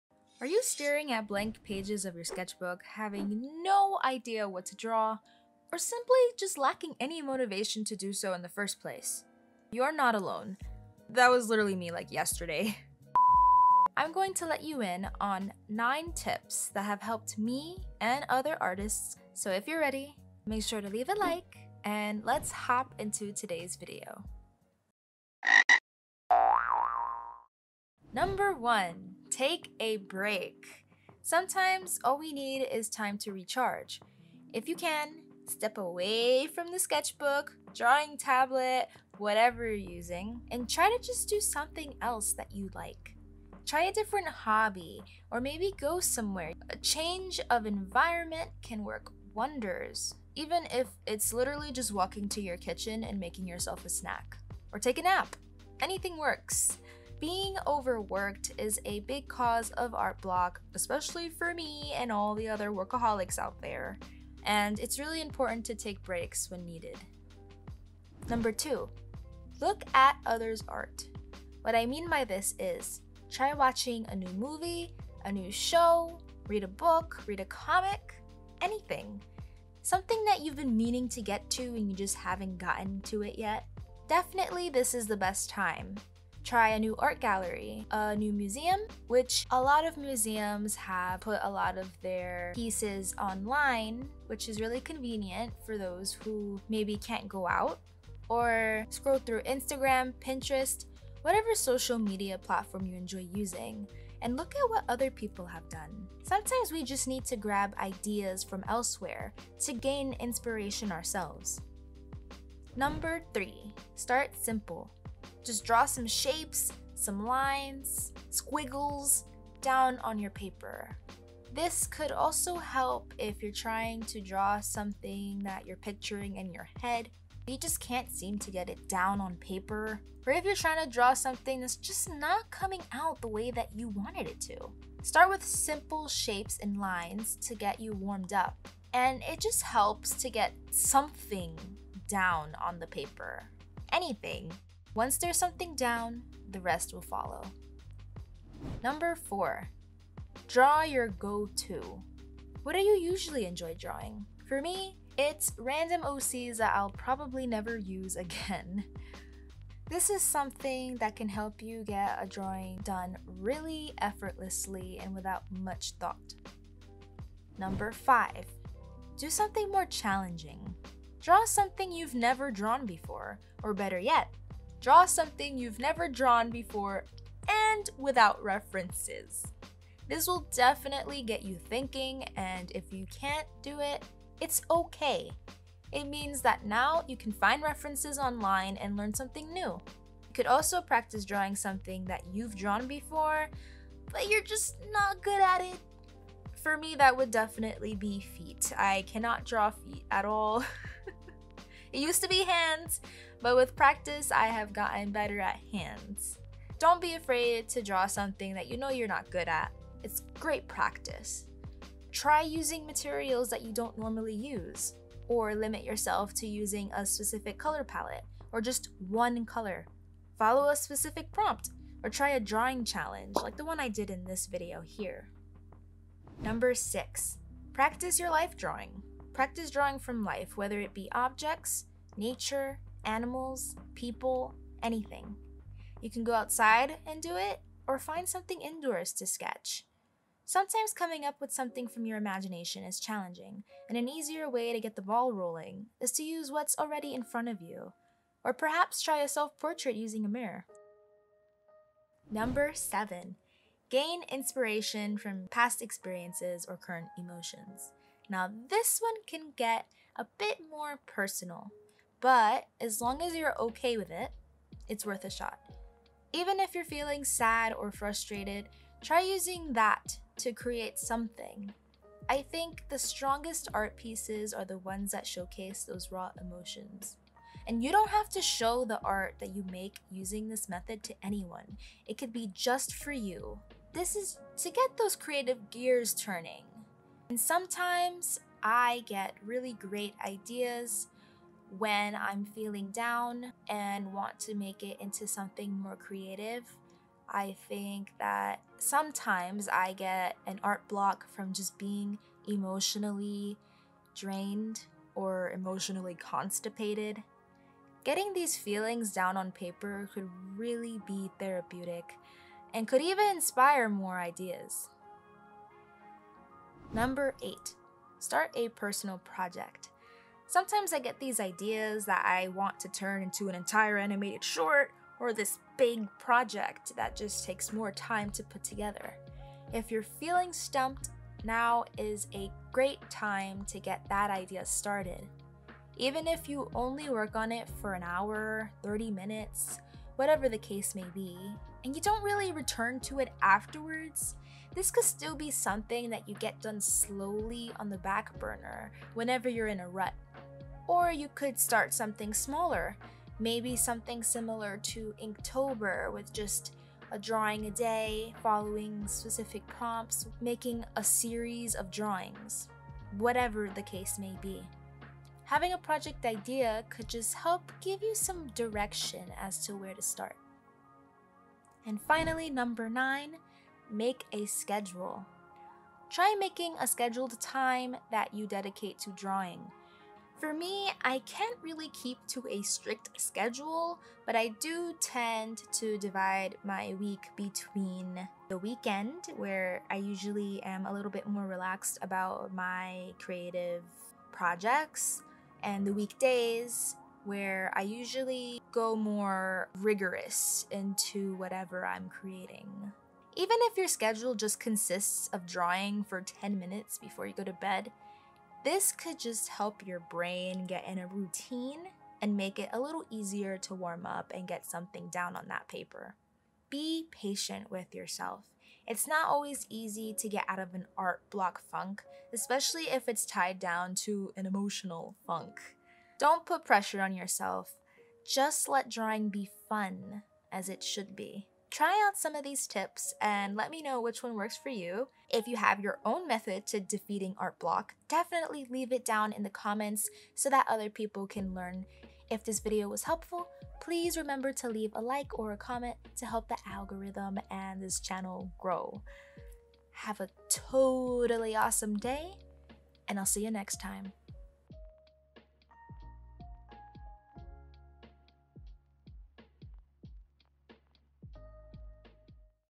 9 Tips to Defeat Art Block
Here are my 9 tips to help you defeat your case of art block. It happens to the best of us, whether it's due to burnout or lacking motivation.

0:00 Can You Relate?
0:28 1.Take a break
1:08 2.Look at other's art
1:52 3.Start simple
2:28 4.Draw Your Go-To
2:47 5.Draw Something Challenging
3:46 6.Practice Life Drawing
4:12 7.Experiences/Emotions
5:16 8.Start a Personal Project
6:12 9.Make a Schedule
6:56 Keep Drawing Fun